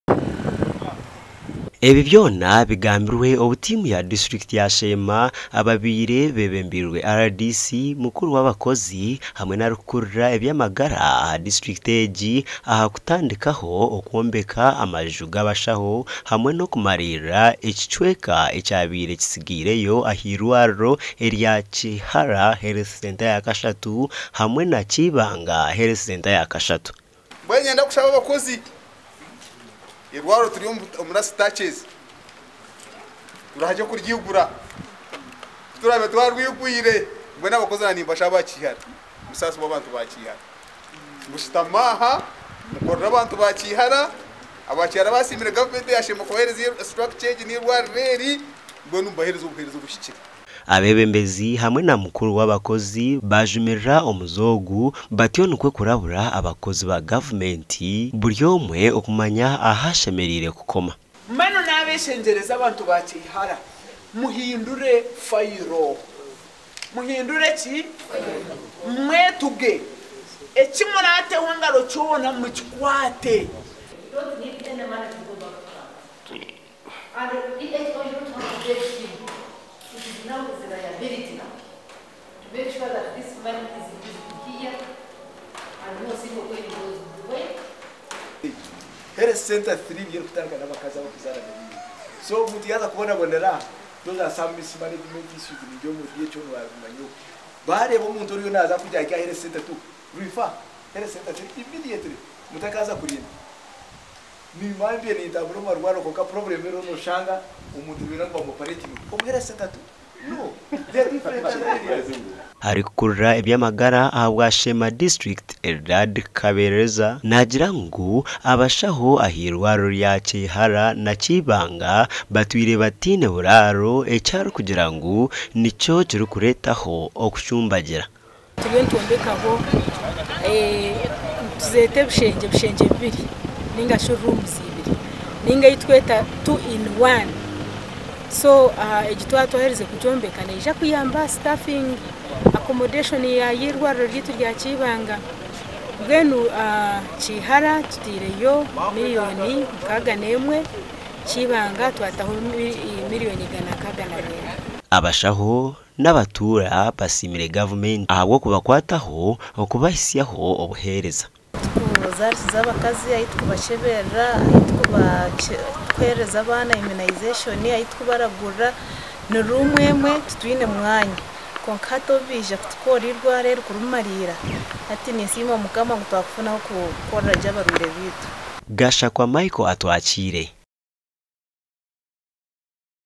AGACENCWIRE: Abatuura ba Kasaana nibetomboita owa mukuru w'abakozi.
Sheema;

Abatuura ba Kasaana nibetomboita owa mukuru w'abakozi.